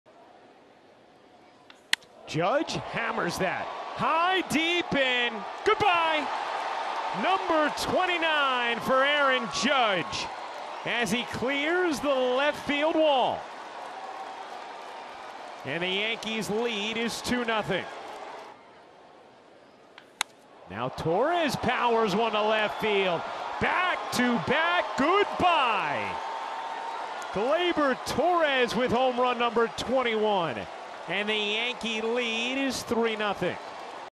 Aaron Judge & Gleyber Torres Slug Back-To-Back 💣💣
Twitter.com/BarnHasSpoken2

Instagram.com/BarnHasSpoken